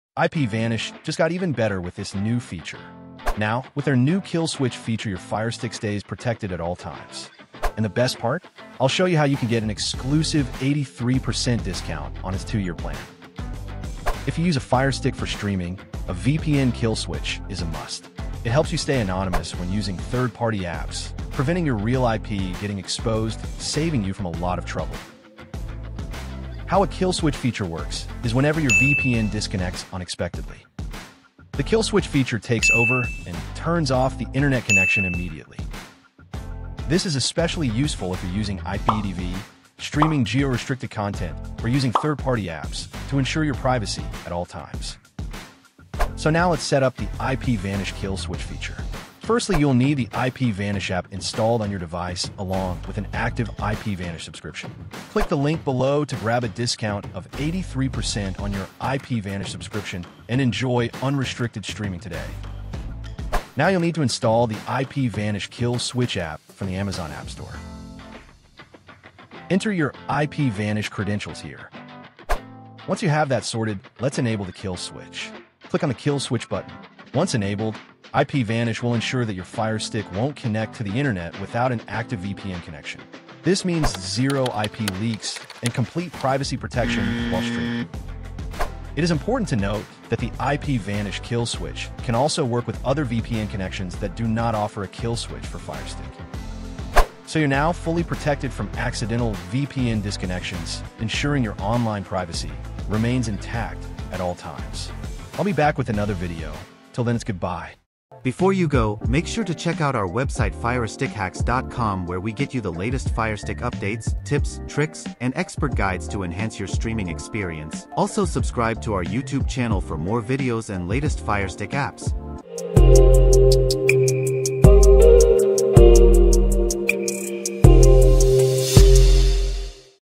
How to Use NEW IPVanish Kill Switch Feature on FireStick
In this video I'll walk you through exactly how to use the new IPVanish Kill Switch feature on your Amazon FireStick. If you want to ensure your streaming activity remains protected, this is a must-watch!

Download All Apps With ONE Downloader Code ➡️ 730116